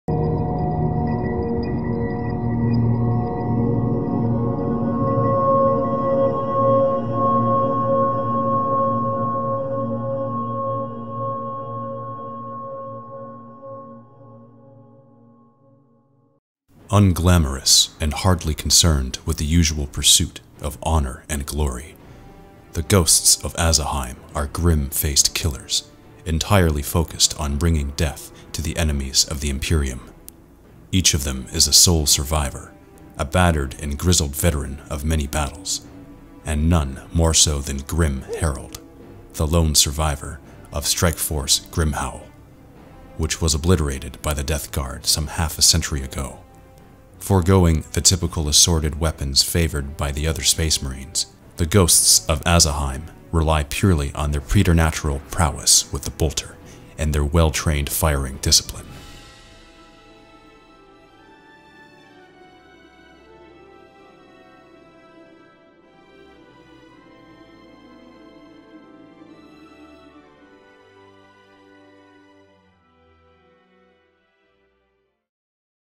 Ghosts OF Aasheim
Teaser for the upcoming Doom of Eolicanth Warhammer 40,000 Space Wolves vs Deathguard narrative campaign.